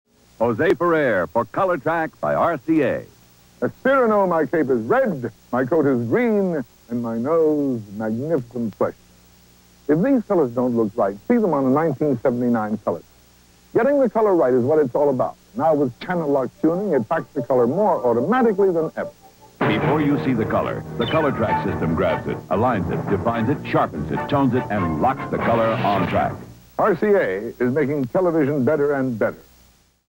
RCA ColorTrak w/ José Ferrer
In this spot, José Ferrer reprises an Oscar winning role of his to sell RCA ColorTrak TV's. Correct me if I'm wrong, but I think that this is a color kinescope of the ad. The original commercial was on tape.